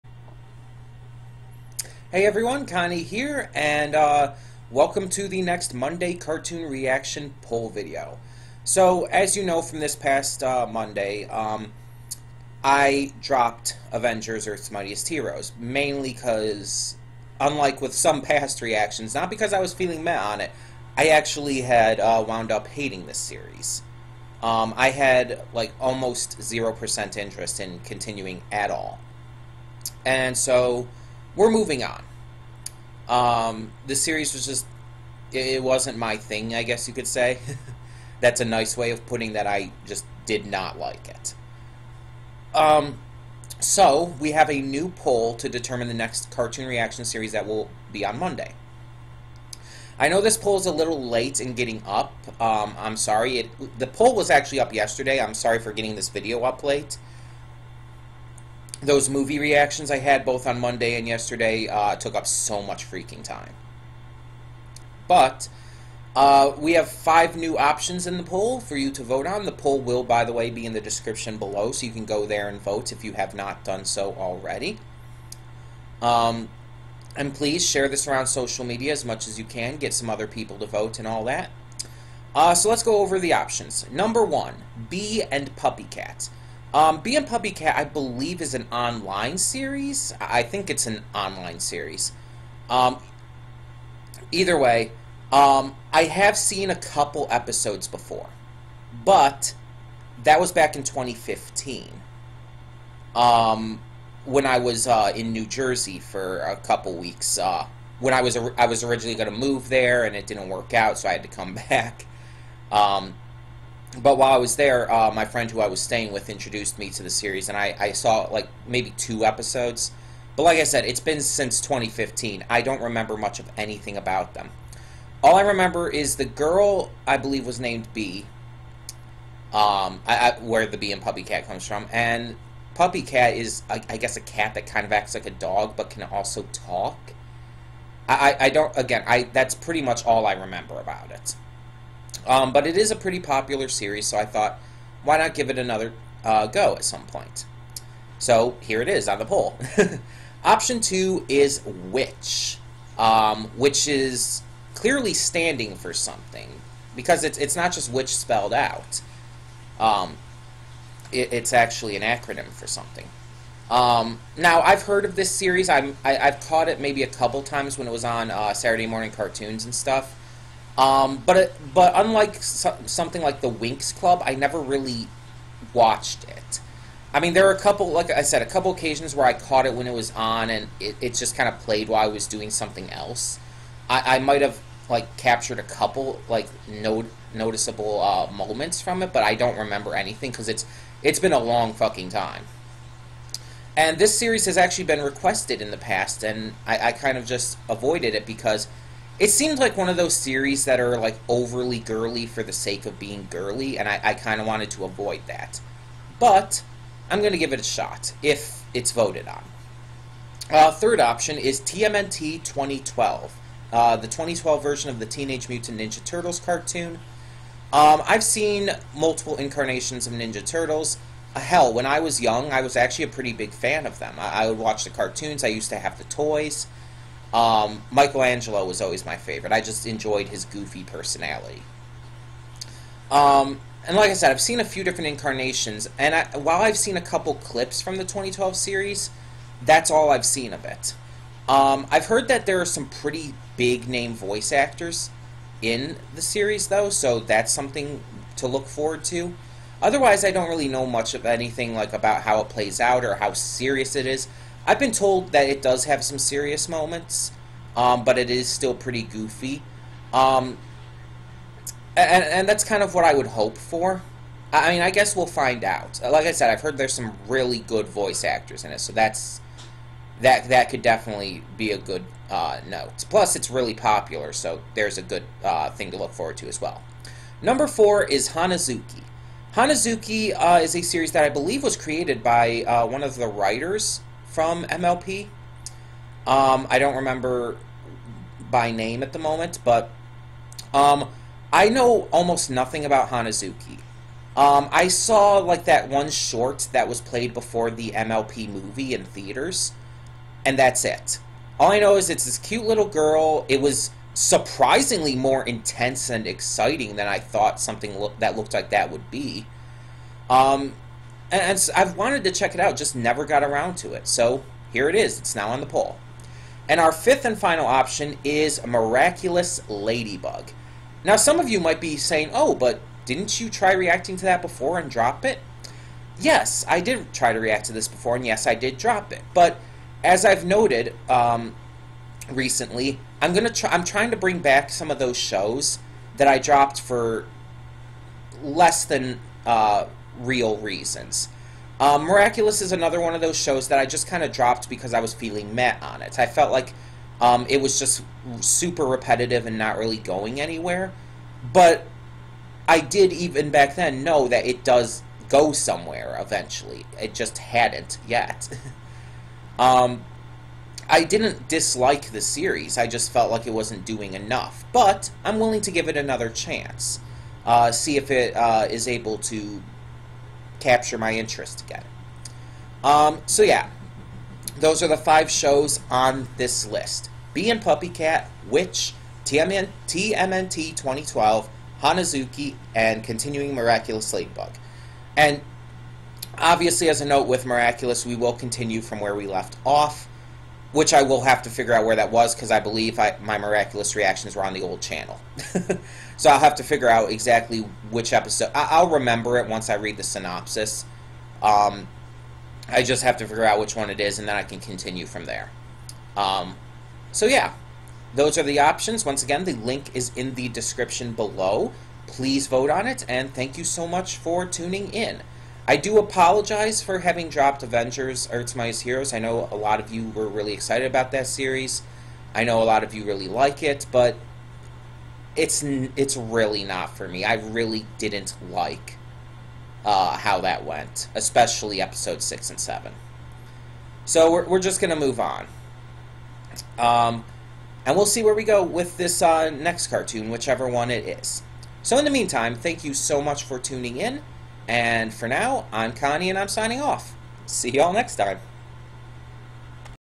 NEW MONDAY CARTOON REACTION POLL
It's time to choose our next reaction series for Mondays. This time we're going for a set of five fun, cute, and generally silly shows in comparison to some of the other stuff we've checked out.

Thumbnail Image: Update video
Thumbnail Design: Connie Royal